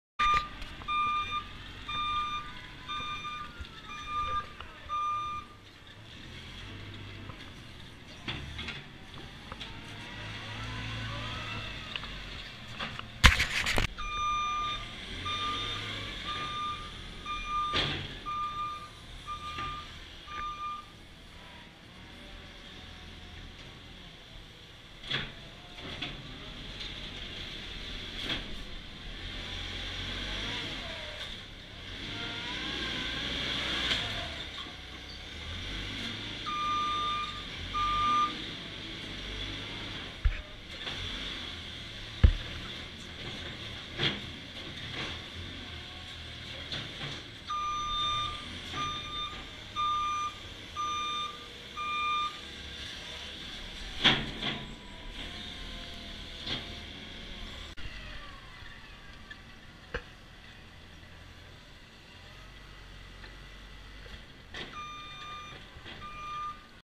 mucking out
mucking out the cows with the Massey Ferguson telehandler